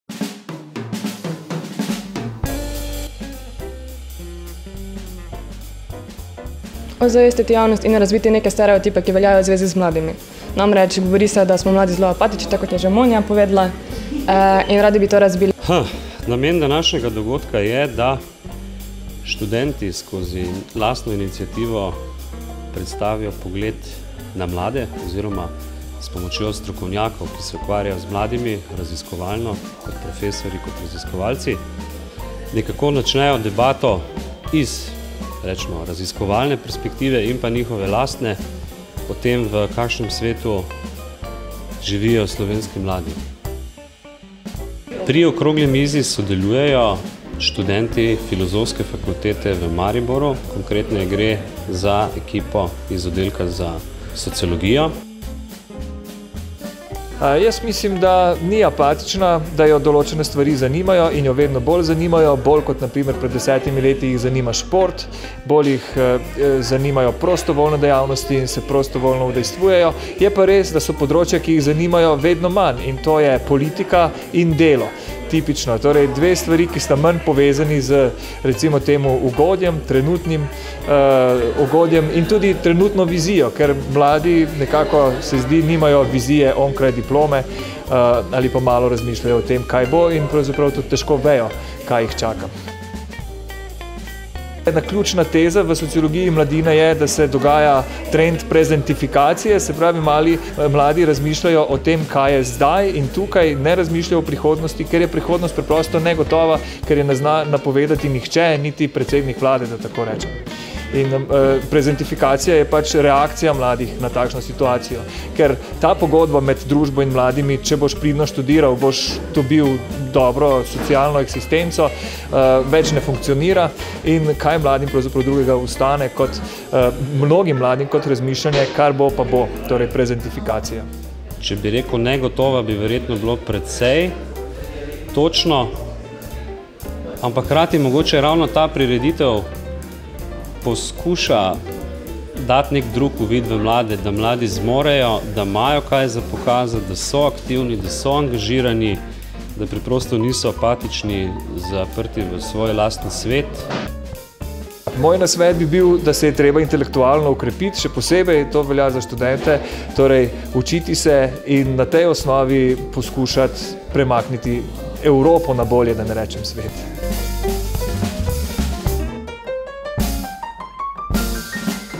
ŠOK - Na mladih svet stoji. Na čem pa stojijo mladi?
Hiša znanosti | 30. maj 2012 | 18:00

Kakšna je danes mladina? Ali jo čaka prihodnost? Ali je zares apatična?

Prisluhnite okrogli mizi študentov Filozofske fakultete Univerze v Mariboru, Oddelka za sociologijo. Pridružili so se jim dr. Miran Lavrič, sociolog in vodja raziskave Mladina 2010, dr. Andrej Naterer, sociolog, antropolog in sodelujoči v raziskavi Mladina 2010 ter dr. Vesna Vuk Godina, najbolj znana slovenska antropologinja in aktivna kolumnistka.